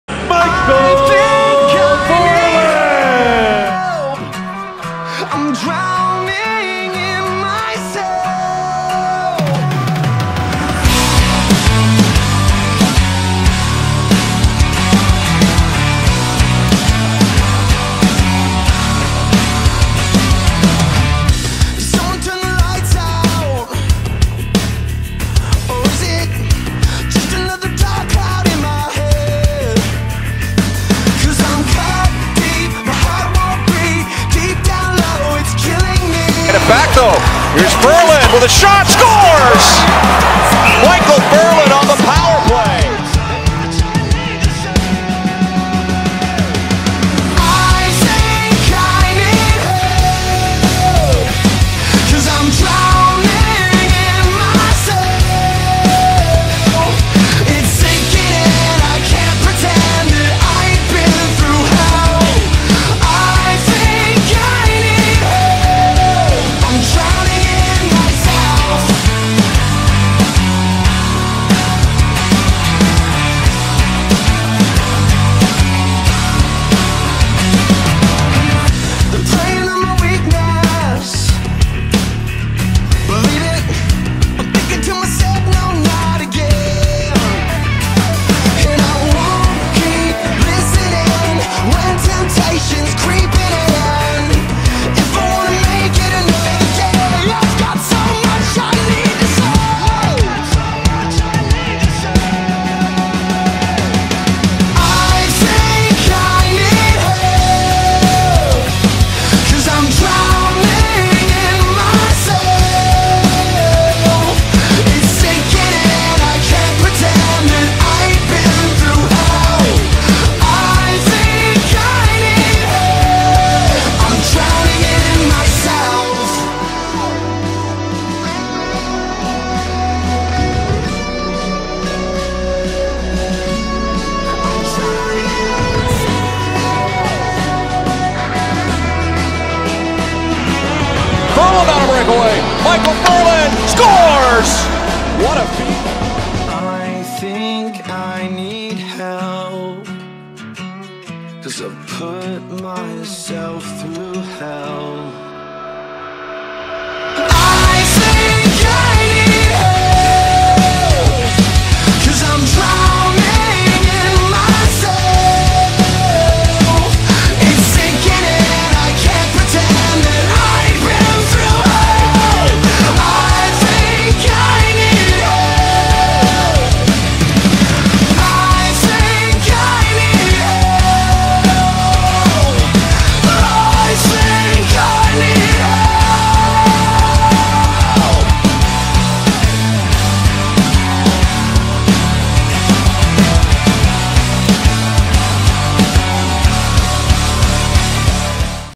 Michael Ferland Montage | 2016-2017
Relieve some of the moments from Number 79 of the Calgary Flames from the 16/17 Season.